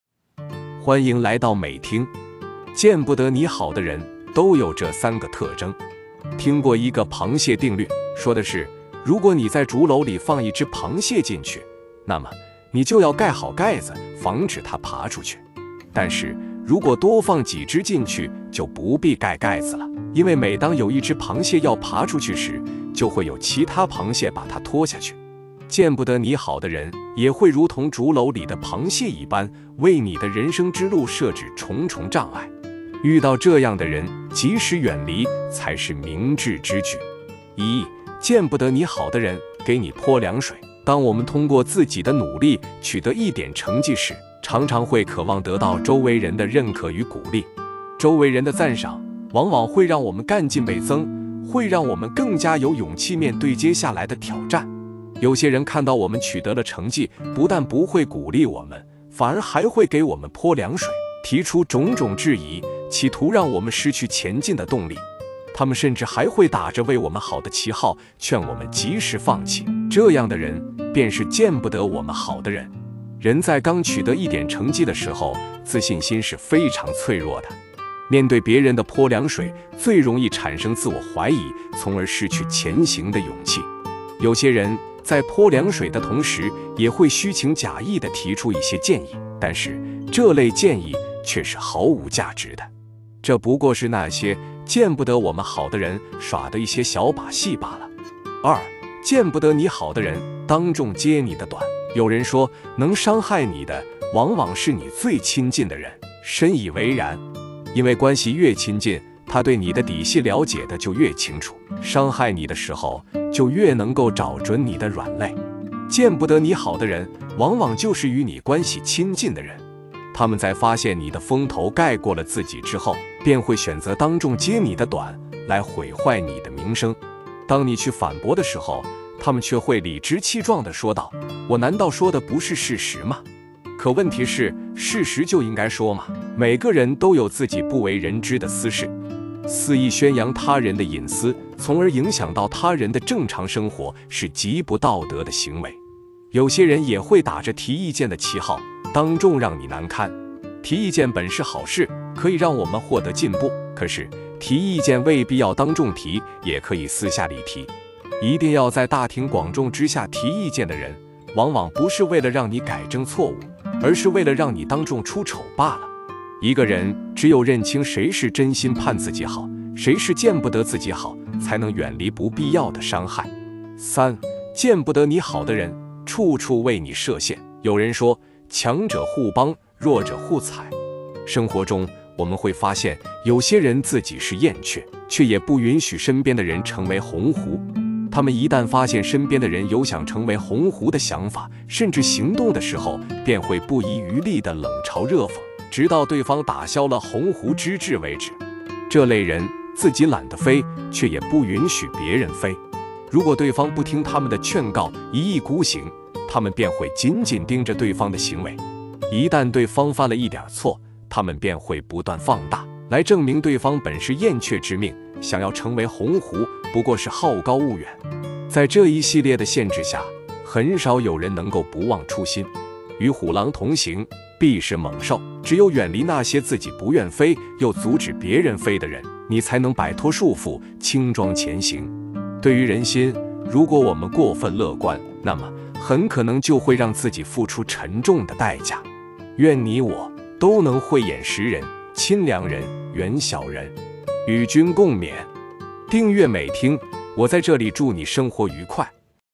見不得你好的人，平時對你都有這三個特征
聽過一個「螃蟹定律」。

說的是，如果你在竹簍裏放一只螃蟹進去，那麽，你就要蓋好蓋子，防止它爬出去。

但是，如果多放幾只進去，就不必蓋蓋子了。

因為每當有一只螃蟹要爬出去時，就會有其他螃蟹把它拖下去。

見不得你好的人，也會如同竹簍裏的螃蟹一般，為你的人生之路設置重重障礙。

遇到這樣的人，及時遠離，才是明智之舉。